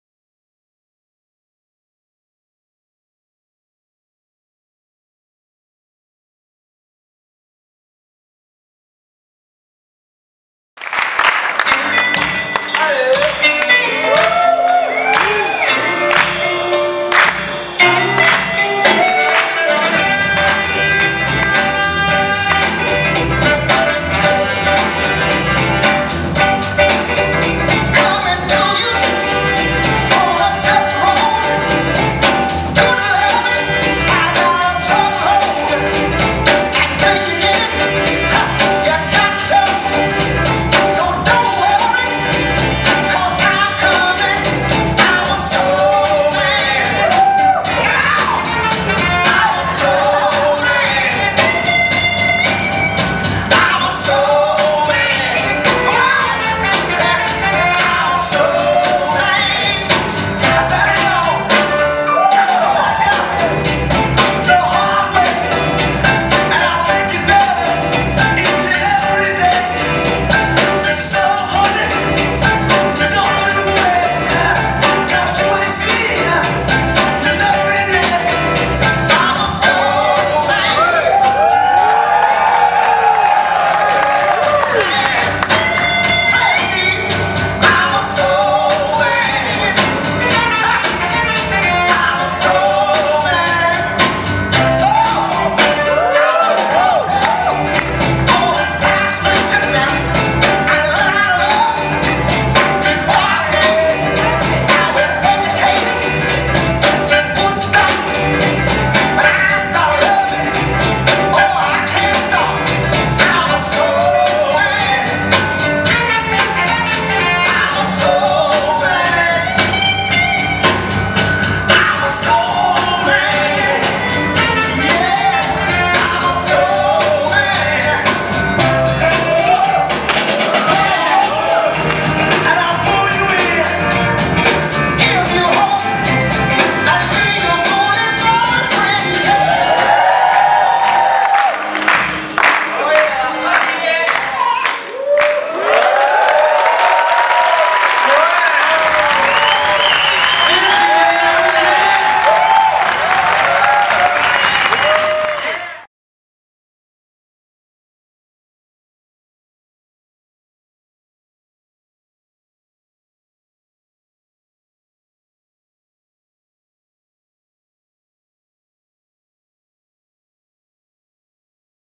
WIC 2019 - Virginie & Olivier Massart
Lead & Follow from Virginie Massart and Olivier Massart
during the dance event
West In Champagne 2019.
West In Champagne is the creation of West Coast Swing en Champagne association, non profit organization to make U dance